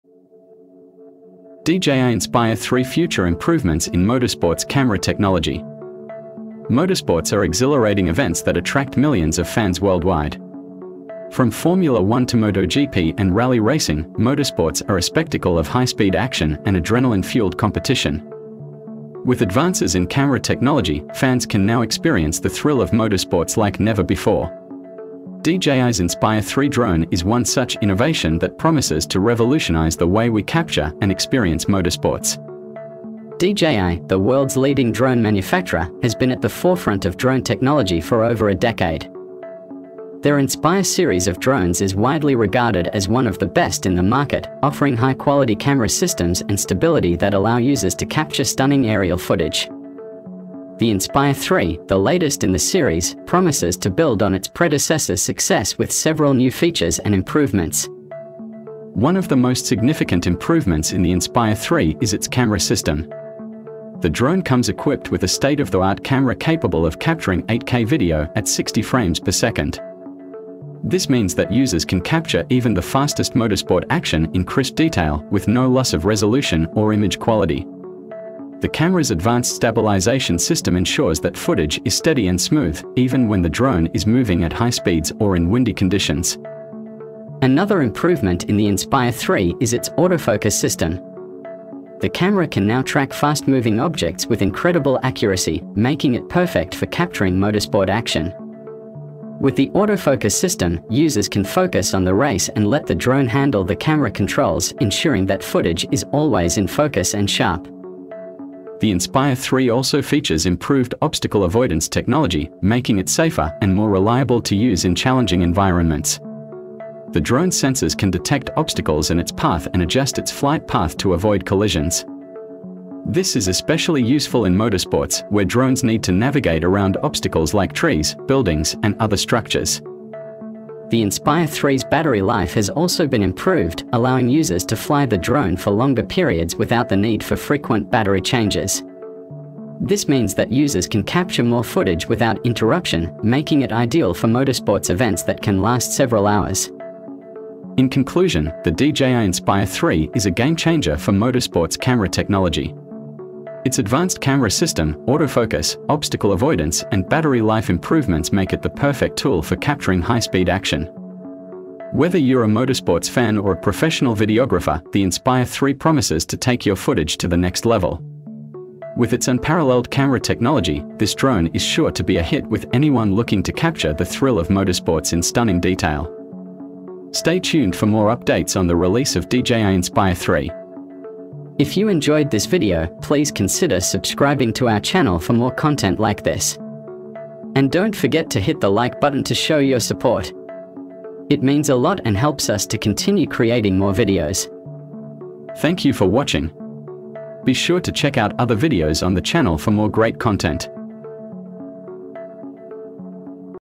DJI Inspire 3 - Professional Features for Motorsport - Part 3 - Camera features
DJI Inspire 3 Future Improvements in Motorsports Camera Technology.

Motorsports are exhilarating events that attract millions of fans worldwide. From Formula 1 to MotoGP and rally racing, motorsports are a spectacle of high-speed action and adrenaline-fueled competition. With advances in camera technology, fans can now experience the thrill of motorsports like never before. DJI's Inspire 3 drone is one such innovation that promises to revolutionize the way we capture and experience motorsports.

DJI, the world's leading drone manufacturer, has been at the forefront of drone technology for over a decade. Their Inspire series of drones is widely regarded as one of the best in the market, offering high-quality camera systems and stability that allow users to capture stunning aerial footage. The Inspire 3, the latest in the series, promises to build on its predecessor's success with several new features and improvements.

One of the most significant improvements in the Inspire 3 is its camera system. The drone comes equipped with a state-of-the-art camera capable of capturing 8K video at 60 frames per second. This means that users can capture even the fastest motorsport action in crisp detail, with no loss of resolution or image quality. The camera's advanced stabilization system ensures that footage is steady and smooth, even when the drone is moving at high speeds or in windy conditions.

Another improvement in the Inspire 3 is its autofocus system. The camera can now track fast-moving objects with incredible accuracy, making it perfect for capturing motorsport action. With the autofocus system, users can focus on the race and let the drone handle the camera controls, ensuring that footage is always in focus and sharp.

The Inspire 3 also features improved obstacle avoidance technology, making it safer and more reliable to use in challenging environments. The drone's sensors can detect obstacles in its path and adjust its flight path to avoid collisions. This is especially useful in motorsports, where drones need to navigate around obstacles like trees, buildings, and other structures.

The Inspire 3's battery life has also been improved, allowing users to fly the drone for longer periods without the need for frequent battery changes. This means that users can capture more footage without interruption, making it ideal for motorsports events that can last several hours.

In conclusion, the DJI Inspire 3 is a game-changer for motorsports camera technology. Its advanced camera system, autofocus, obstacle avoidance, and battery life improvements make it the perfect tool for capturing high-speed action. Whether you're a motorsports fan or a professional videographer, the Inspire 3 promises to take your footage to the next level. With its unparalleled camera technology, this drone is sure to be a hit with anyone looking to capture the thrill of motorsports in stunning detail.